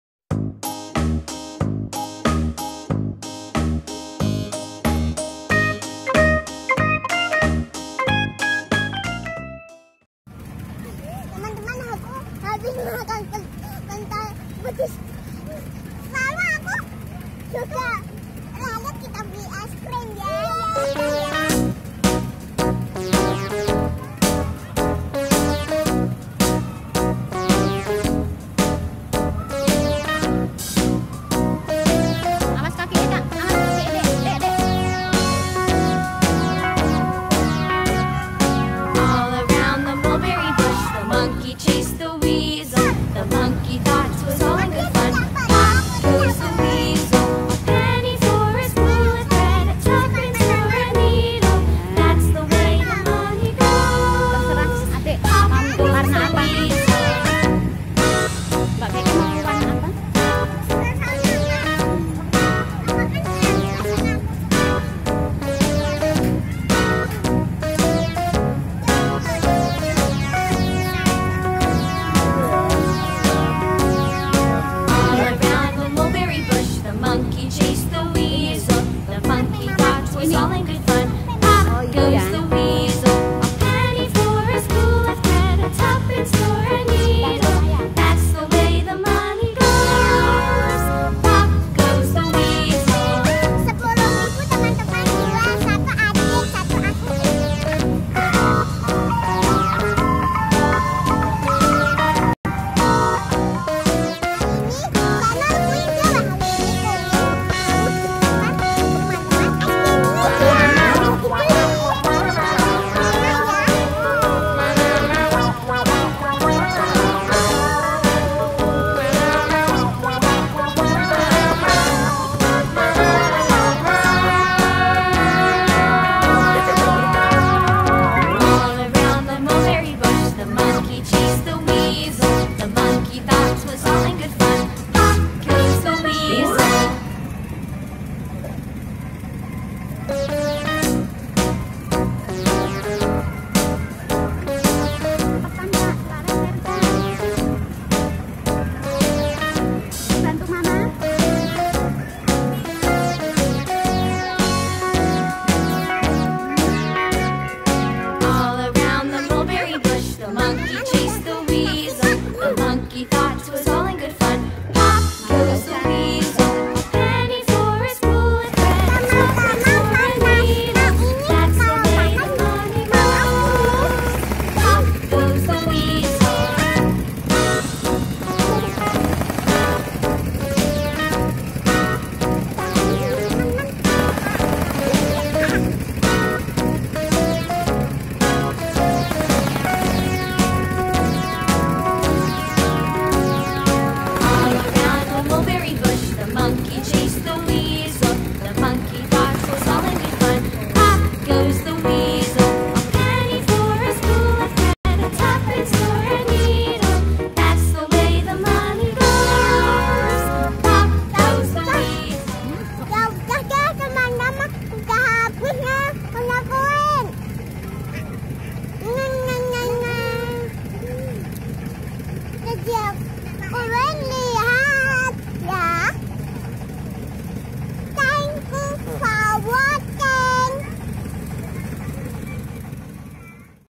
Balita Lucu makan Es Krim di Pasar Malam💗di buang sayang
Assalamualaikum teman-teman... Tonton keseruan Kekey Quinn makan es krim saat berada di Pasar Malam..si Quinn lucu kocak seru sekali... thankyuporwocing hehe...